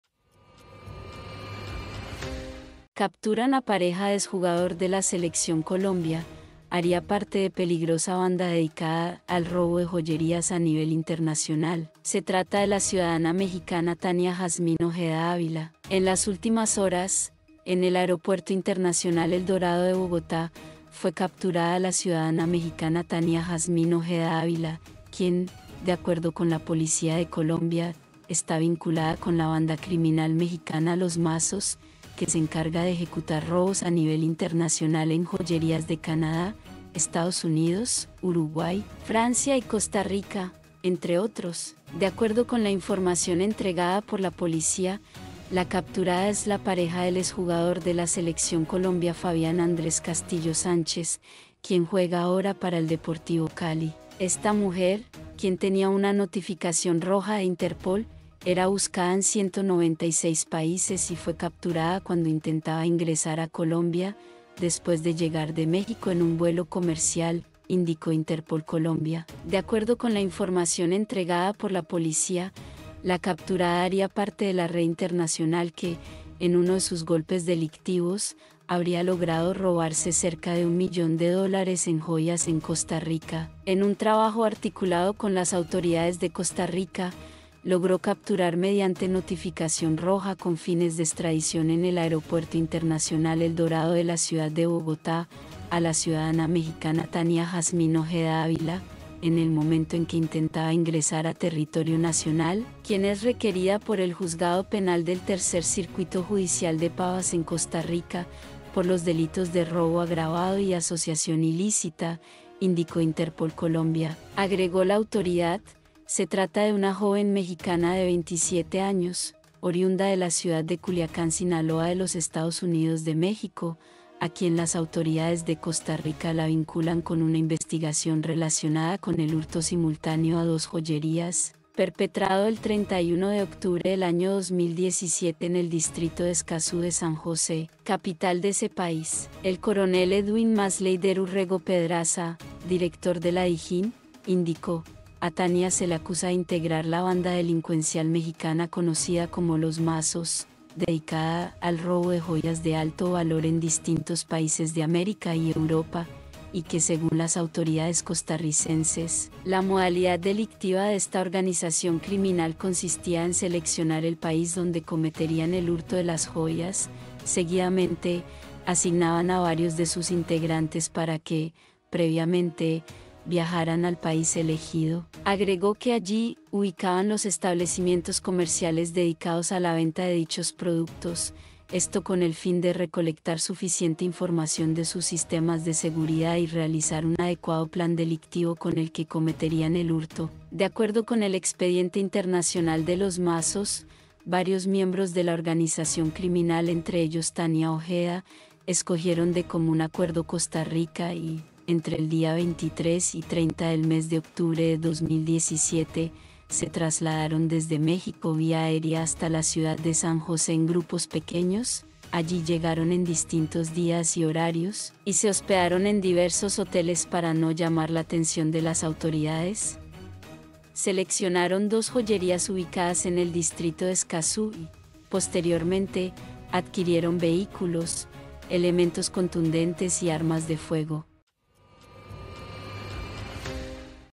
URGENTE ⛔ Capturan a pareja de exjugador de la Selección Colombia
Capturan a pareja de exjugador de la Selección Colombia; haría parte de peligrosa banda dedicada al robo de joyerías a nivel internacional
Se trata de la ciudadana mexicana Tania Jazmín Ojeda Ávila.
SEMANA